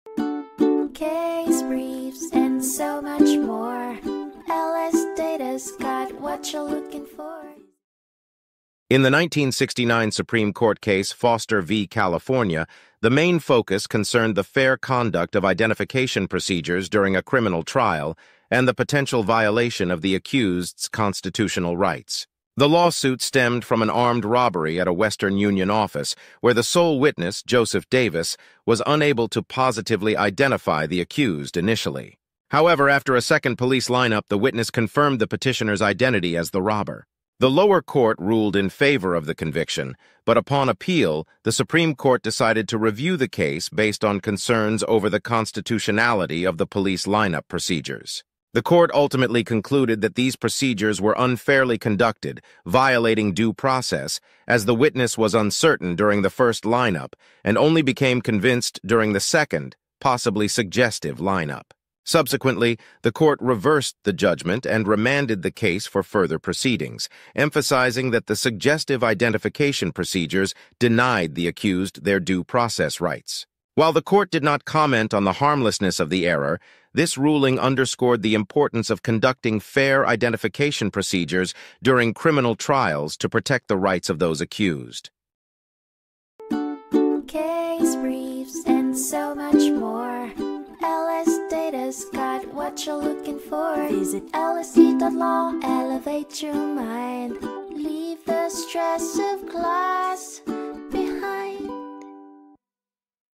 Foster v. California (1969) Overview | LSData Case Brief Video Summary
The case is about a person charged with armed robbery at a Western Union office. The only witness was the late-night manager of the office. After being arrested, the petitioner was placed in two police lineups, and the witness initially could not identify him. However, after a second lineup, the witness positively identified the petitioner. During the trial, the witness identified the petitioner as the robber. The Court found that the lineup procedures were unfair and violated due process.

Foster v. California (1969)
394 U.S. 440, 22 L. Ed. 2d 402, 89 S. Ct. 1127, 1969 U.S. LEXIS 2050, SCDB 1968-071